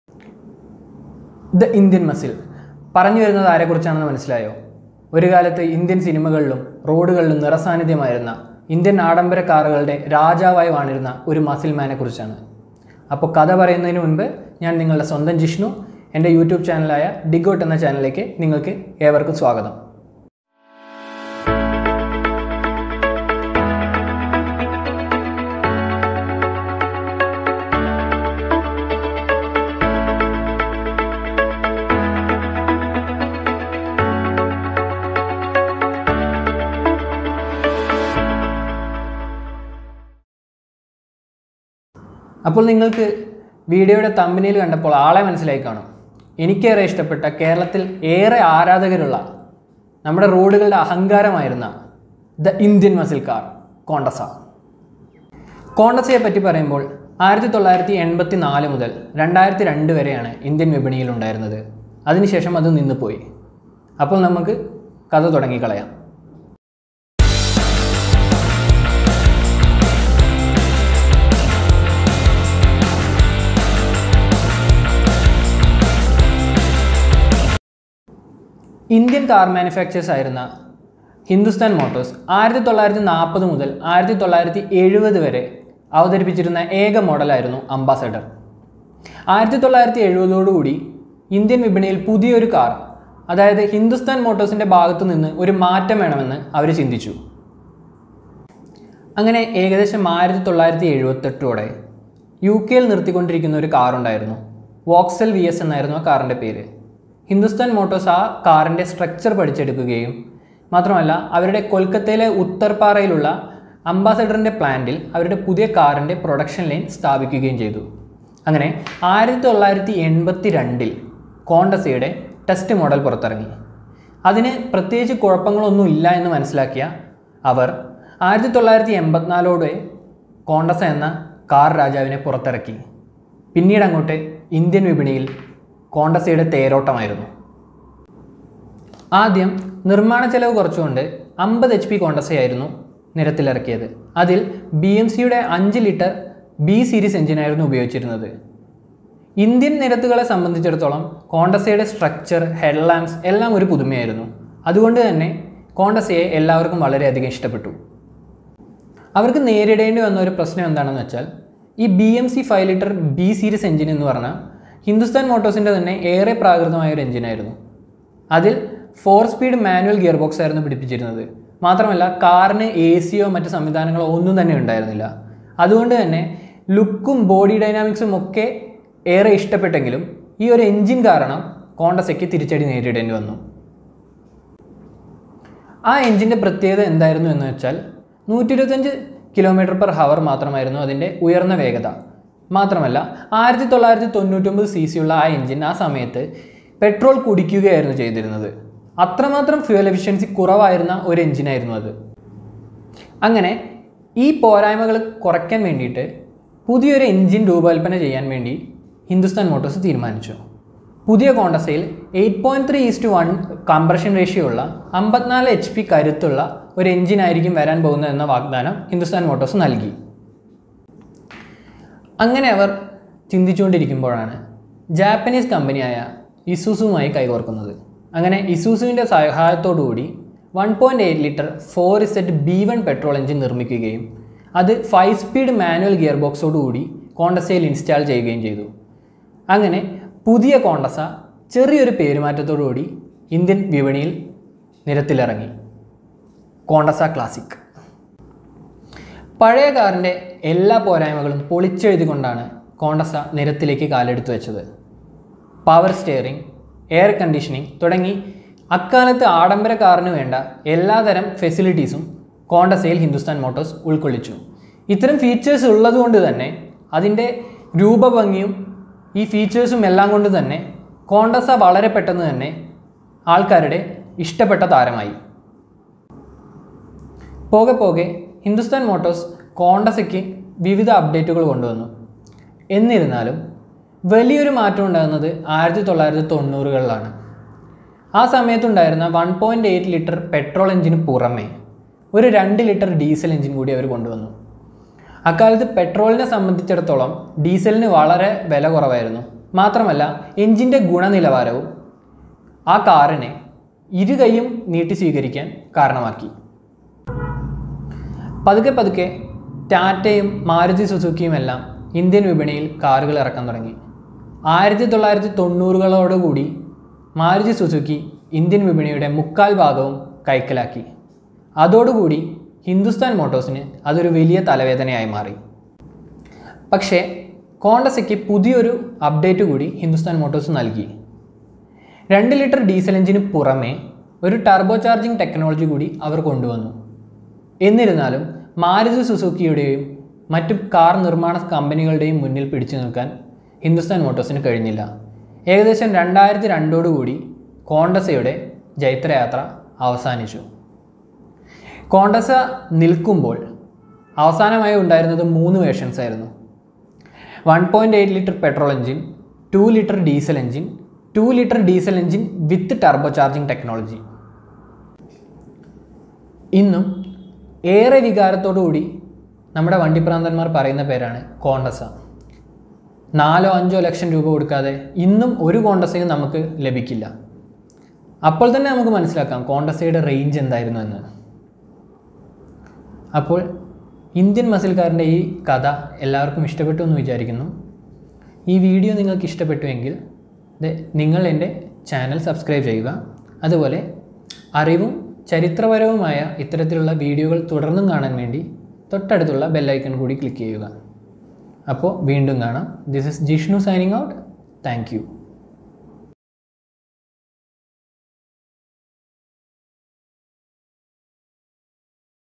Contessa history .......dig out
this video is about the history of contessa.This car is known as indian muscle car. dig out